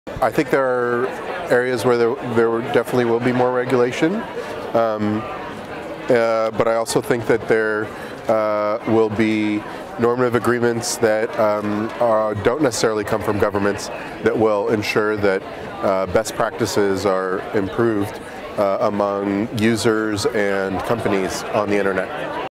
Global IGF 2017: Eddan Katz on Internet Regulation
Eddan Kat, lead of Digital Protocol Network Initiative at Center for the Fourth Industrial Revolution, shares their answer to the question, “Will the Internet become much more regulated in the next 10 years?” Recorded at the Global Internet Governance Forum 2017 in Geneva, Switzerland, by the Imagining the Internet Center, an initiative of Elon University, North Carolina, USA.